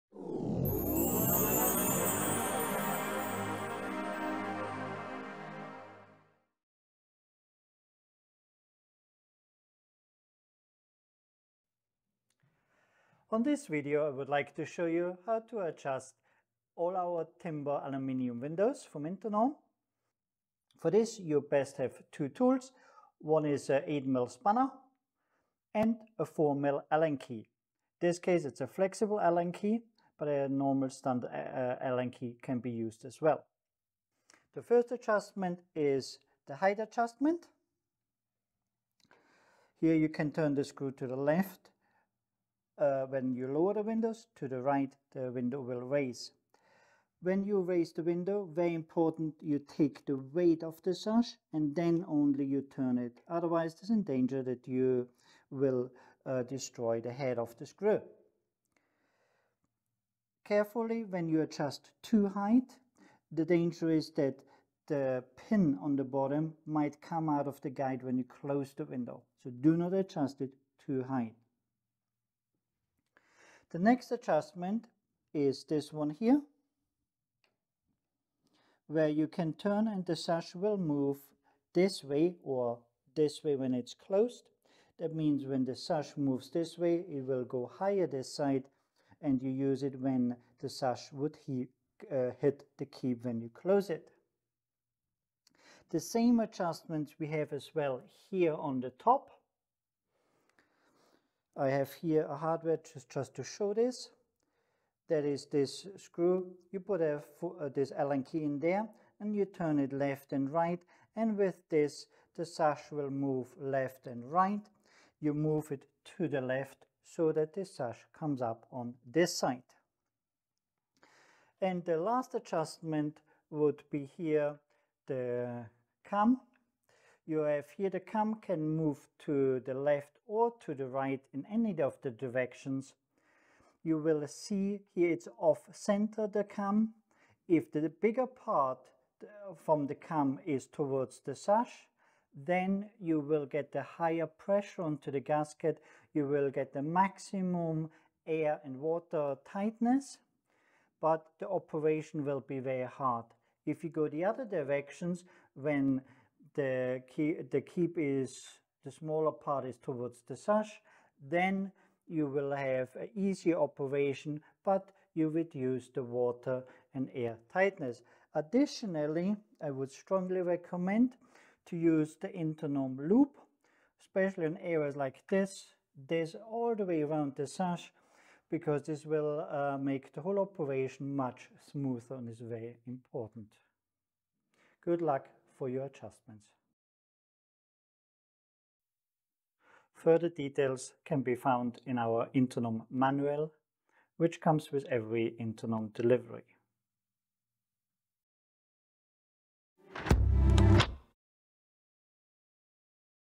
How to adjust an Internorm Timber-Aluminium Composite Window (HF 510, HF 410, HF 310)
Thomas Hagen, Internorm UK Product Manager, explains how to make adjustments to timber-aluminium windows.

This video applies to HF 510 and HF 410, as well HF 310 Internorm timber-aluminium composite windows.